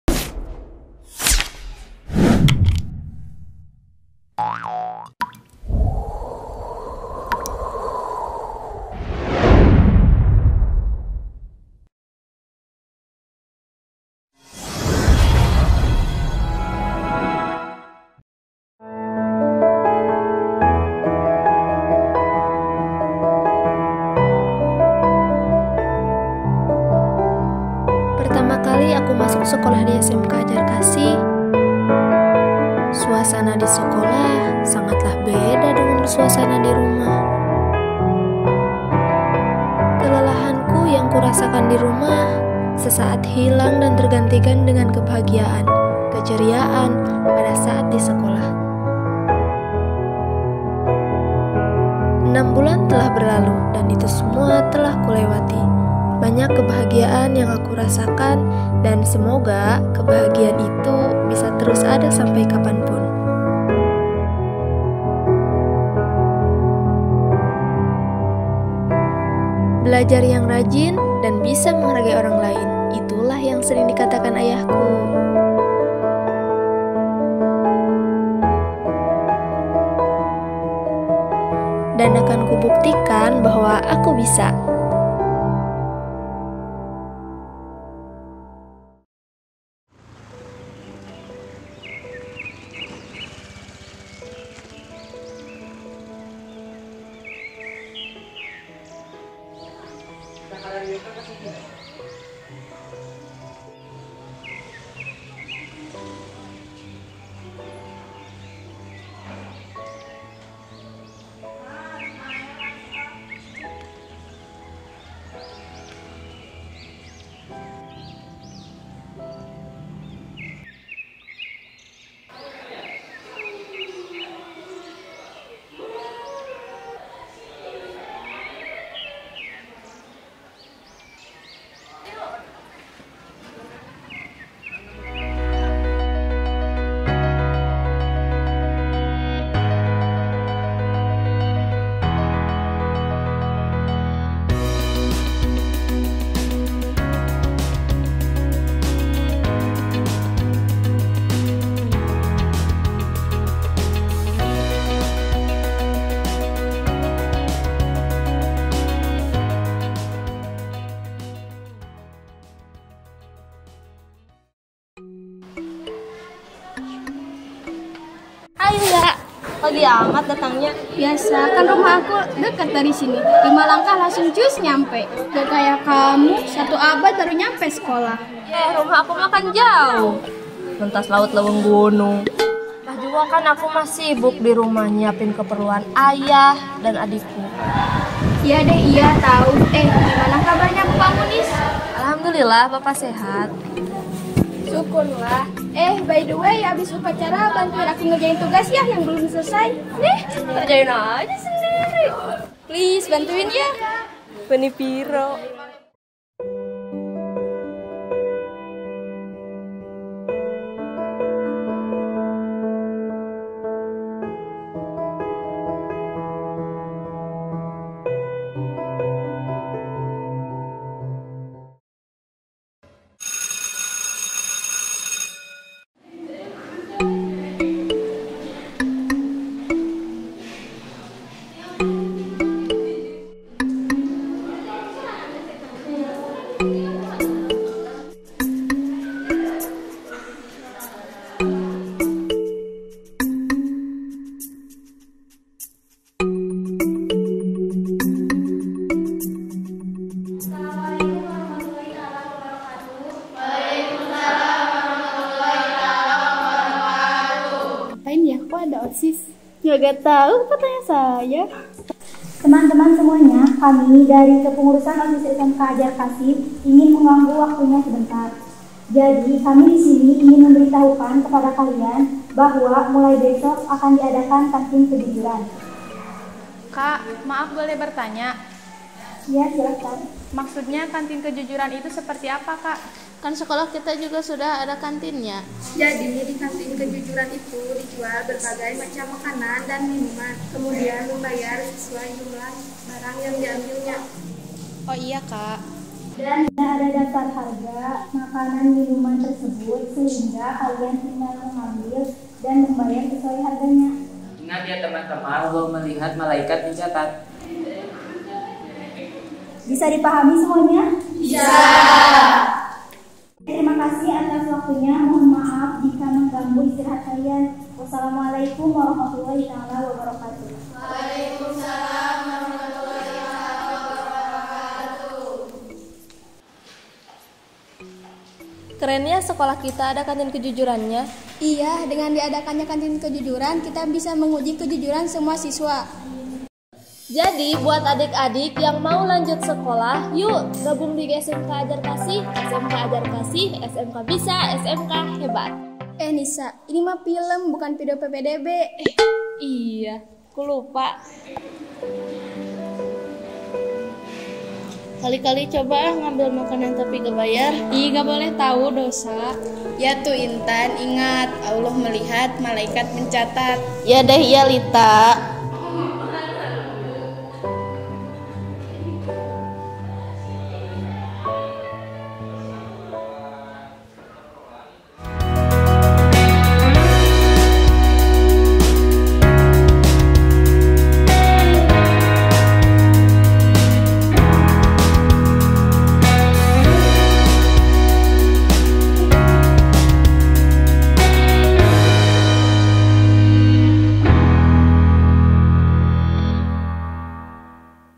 Short Movie - "Allah Melihat, Malaikat Mencatat" || KISAHKU the Series eps.02
Series perdana Produksi SMK AZZ OFFICIAL.
Kisah Ku menceritakan latar belakang dan keseharian siswa siswi di SMK Az-Zarkasyih Leles dari berbagai latar belakang yang berbeda.
Hasil Karya Siswa SMK Az-Zarkasyih Leles 2023 bersama SMK AZZ OFFICIAL Film Productions.

Lebih Jelas nya yu tonton sampai habis.

dan Stay trus pantengin Channel SMK AZZ OFFICIAL.